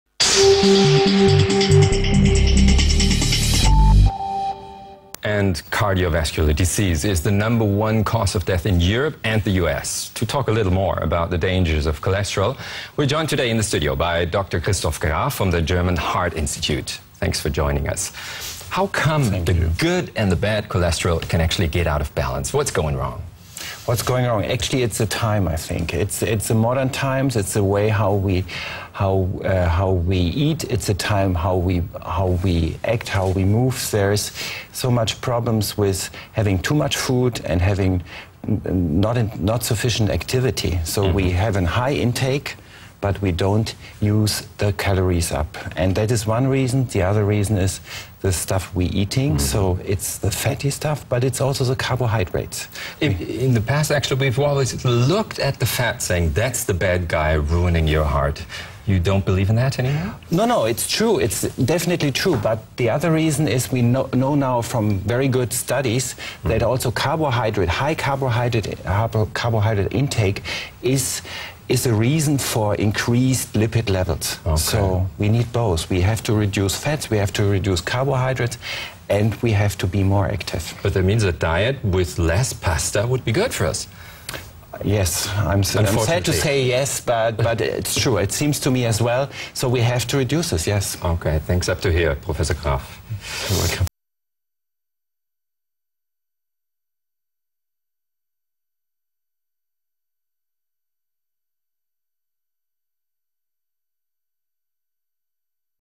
Studio Guest: Prof. Kristof Graf, Cardiologist - Part 1
Prof. Kristof Graf from the German Heart Institute in Berlin talks with TOMORROW TODAY about the cardiovascular disease,the different forms of cholesterol and how they affect the human health system.DW-TV: The Cardiovascular disease is still the number one cause of death in Europe and the US. How can the good and the bad cholestrol actually get out of balance. What's going wrong?
Kristof Graf: What's going wrong? I think,it's our modern times. It's the way we eat,how we act,how we move. There are so many problems with having too much food and not sufficient activity. So we have a high intake but we don't use up the calories. That is one reason. The other reason is the stuff we're eating,so it's the fatty stuff but also the carbohydrates.
DW-TV: In the past actually we've always looked at the fat and said: that's the bad guy ruining your heart. You don't believe in that anymore?
Kristof Graf: No it is true,it's definitely true. But the other reason is,we know now from very good studies that also carbohydrate,high carbohydrate intake,is a reason for increased lipid levels. So we need both - we have to reduce fat,we have to reduce carbohydrates and we have to be more active.
DW-TV: But that means a diet with less pasta would be good for us?
Kristof Graf: Yes unfortunately. I'm sad to say yes,it's true,we have to reduce that.
DW-TV: Thanks up to here. We will talk again,after we had a look at a very special way of preventing the cholesterol danger.
(Interview: Ingolf Baur)